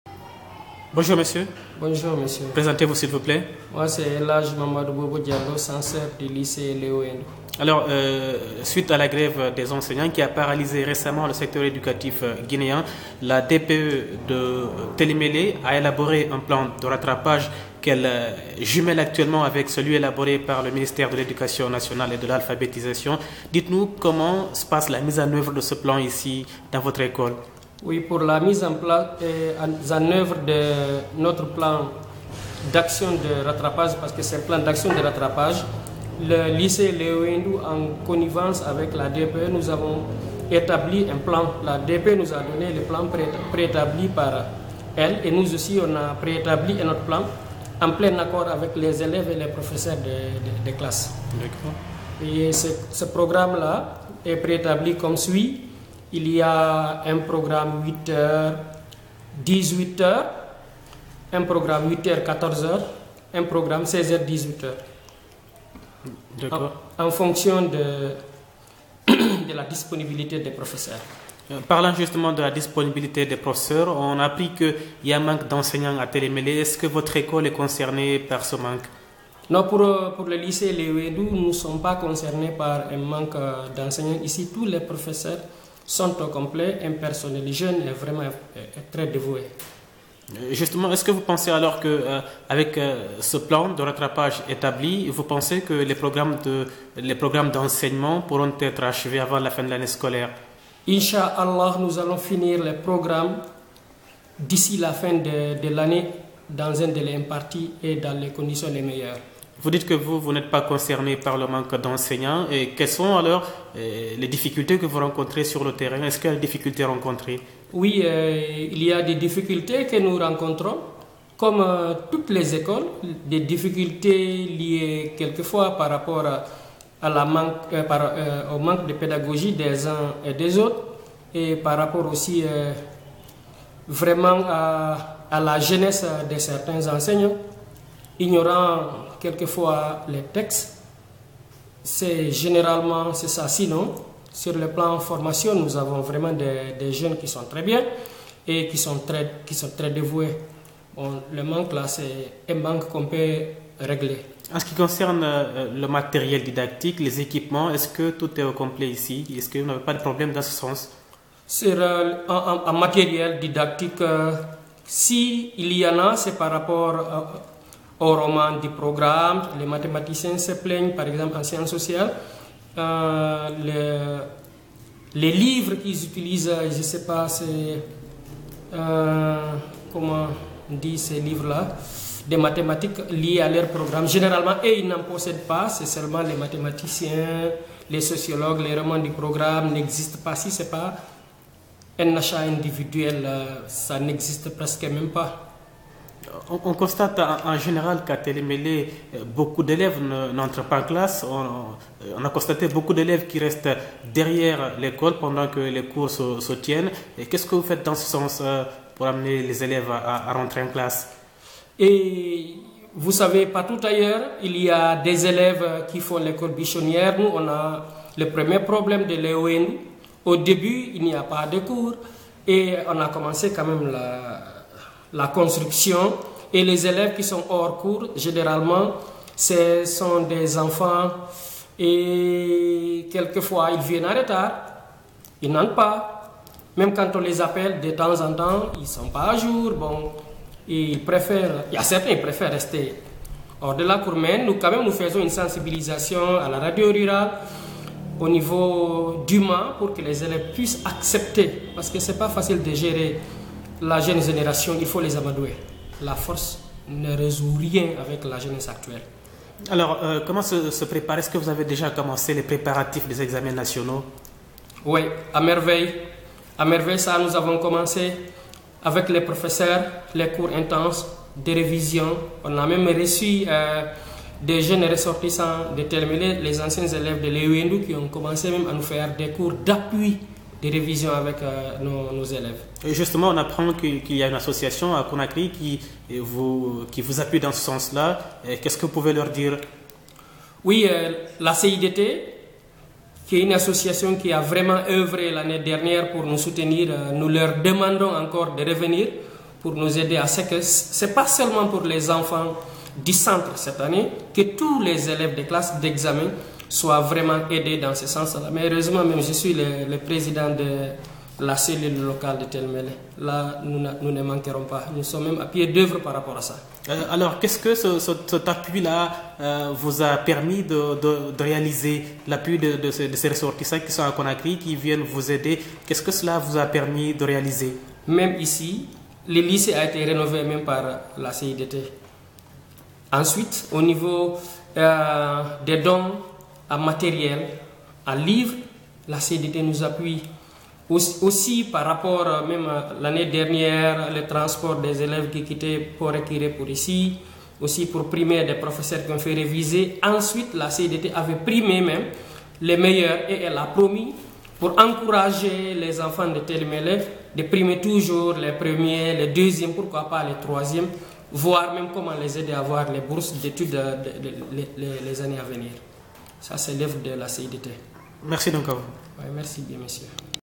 Elhadj Bobo, censeur du Lycée Ley Wéndou, sur le rattrapage des cours à Télimélé
Elhadj Mamadou Bobo, censeur du Lycée Ley Wéndou, sur le rattrapage des cours à Télimélé
"Suite à la suspension de la grève des enseignants, la DPE nous a soumis un plan de rattrapage du temps perdu, nous aussi, au niveau de notre école, on avait élaboré un plan de rattrapage en concertation avec nos enseignants. Il y a un programme 8h-18h, un programme 8h-14h et un programme 16h-18h, en fonction du temps perdu et de la disponibilité des professeurs"